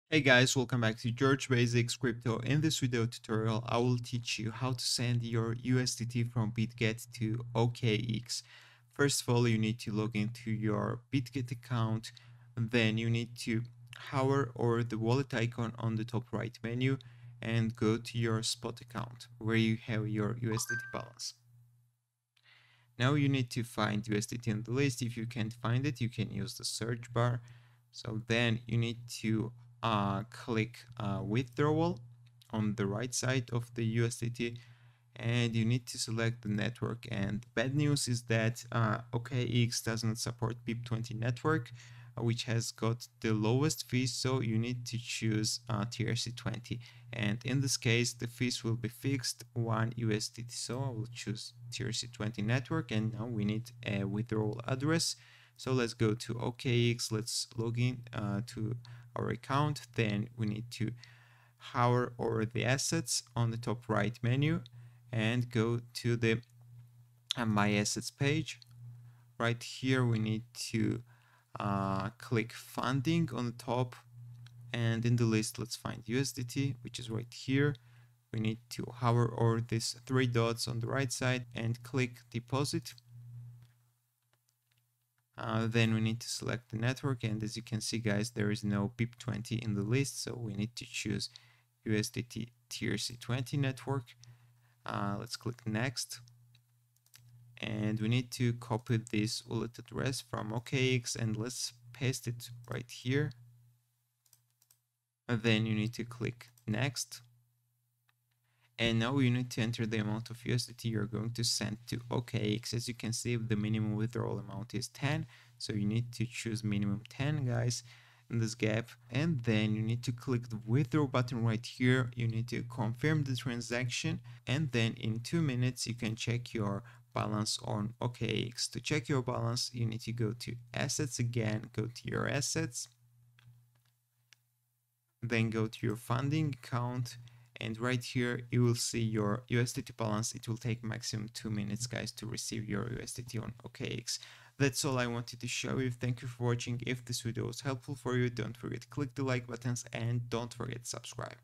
How to Transfer USDT from Bitget to OKX (2026)
How to Transfer USDT from Bitget to OKX (2024)

In this tutorial, we will guide you through the process of transferring USDT (Tether) from your Bitget account to your OKX account. Follow these easy steps to ensure a smooth and secure transaction.

In this video Tutorial, I will teach you:

1. How to send USDT from Bitget to OKX
2. How to Withdraw USDT From Bitget to OKX
3. How to Transfer Crypto From Bitget to OKX